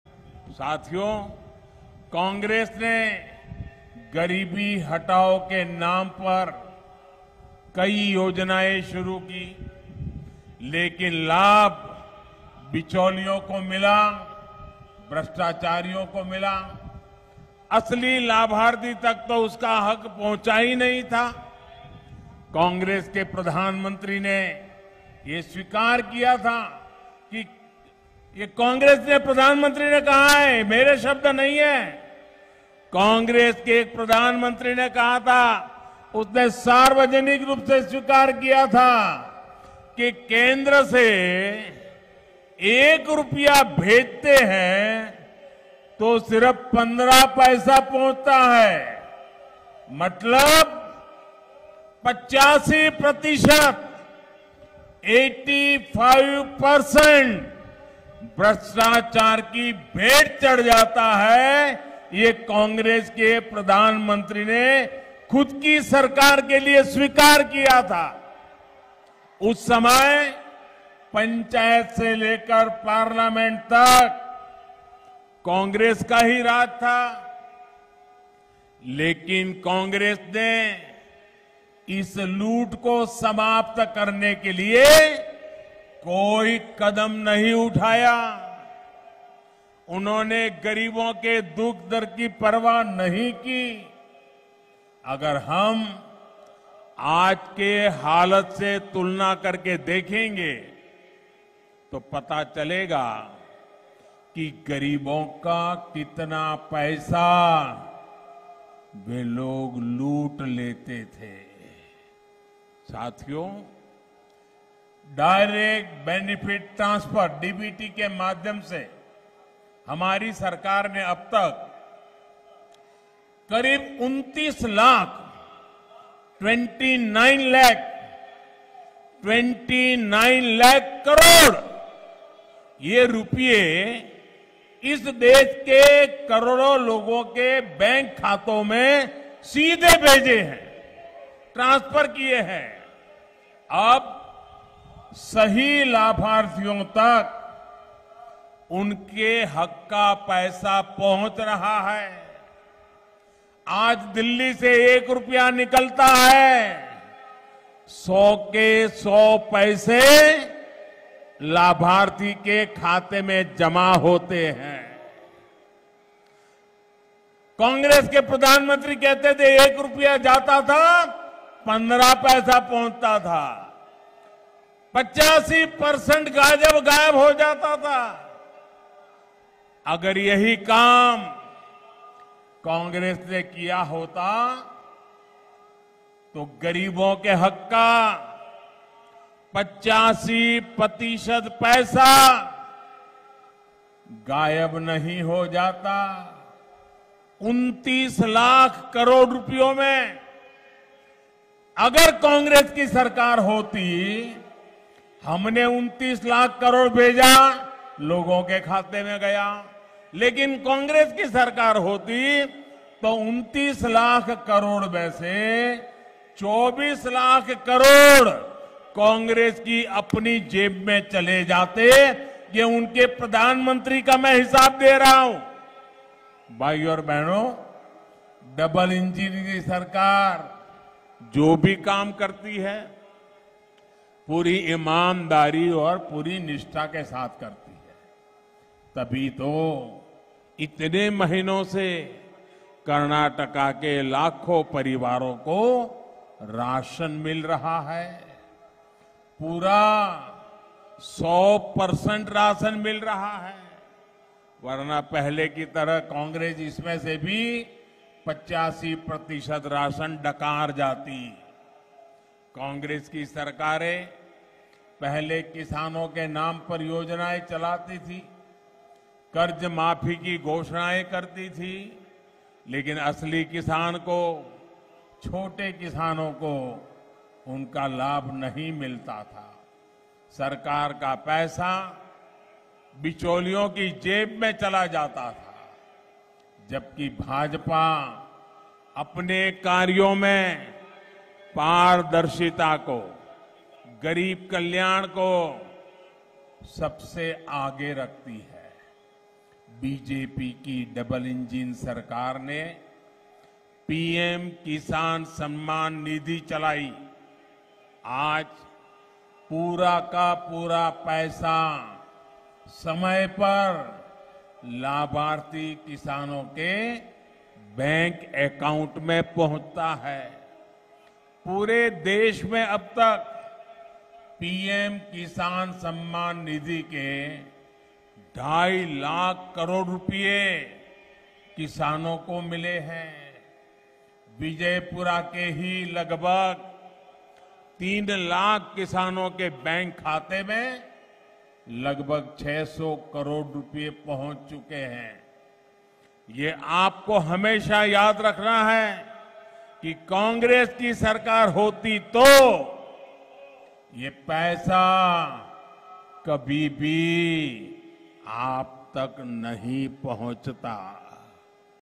अगर Congress की सरकार होती तो कल्याणकारी योजनाओं की 85% राशि Corruption की भेंट चढ़ जाती!
During his second rally in Vijayapura, PM Modi discussed his government's efforts to improve the lives of thousands of Banjara people in Karnataka. He said, “Thousands of families have been given hakku patras. After getting those rights letters, Banjara mothers and sisters were seen confident that now their children will not have to be a victim of neglect, they will not have to live the life of the underprivileged.” The Prime Minister further highlighted the long-standing neglect of the Banjara and Lambani community by the Congress governments, emphasizing that his government was committed to uplifting all sections of society.